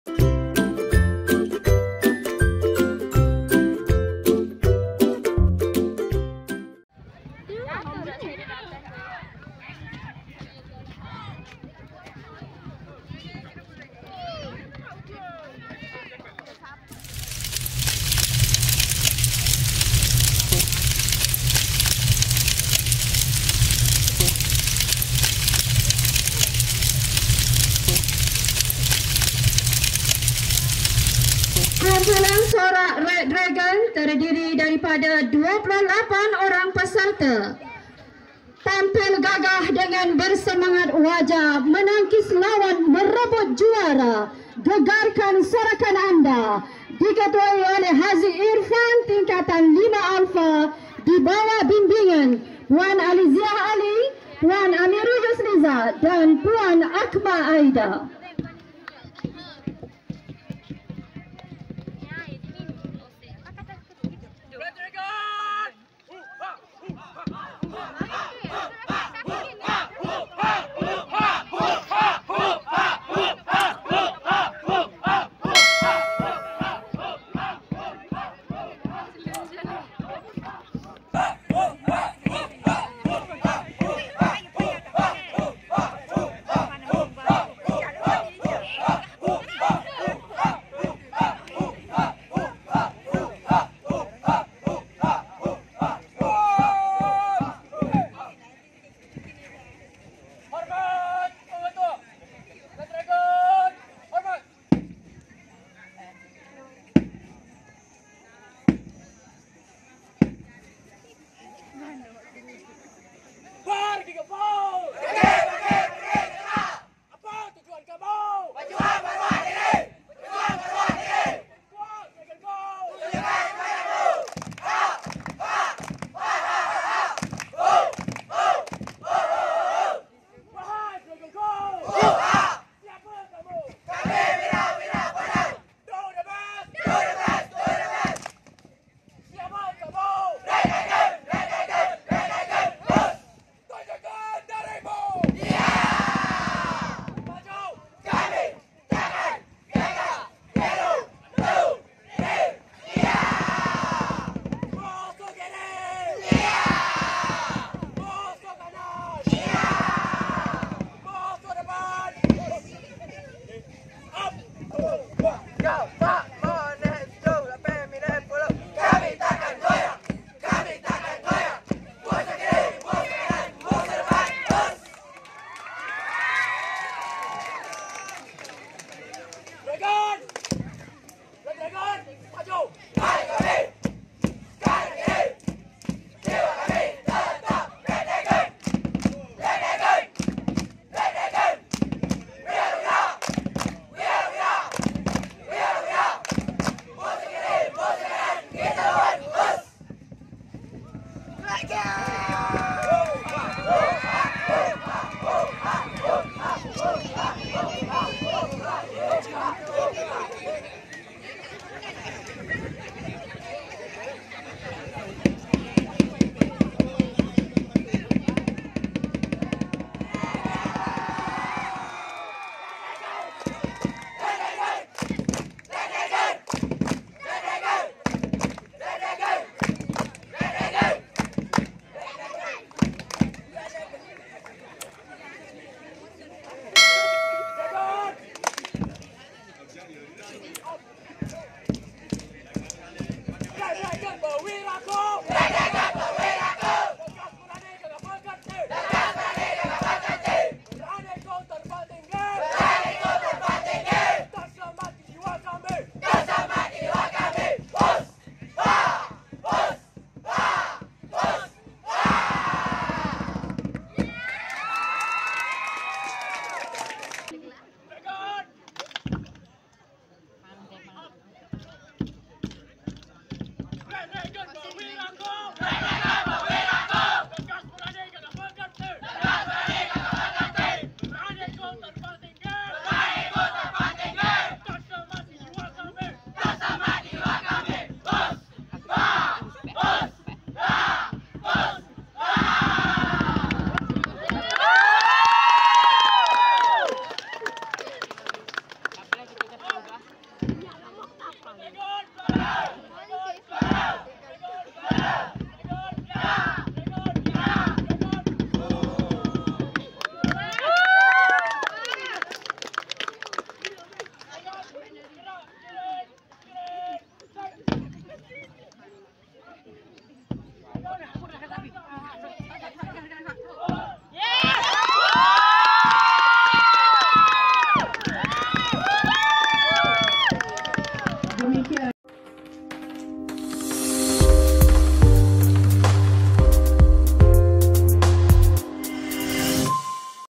Pasukan Sorak Red Dragon - Wakafians
Persembahan pasukan sorak Red Dragon di Temasya Olahraga Wakafians SMK Wakaf Bharu Kali Ke-28 Tahun 2022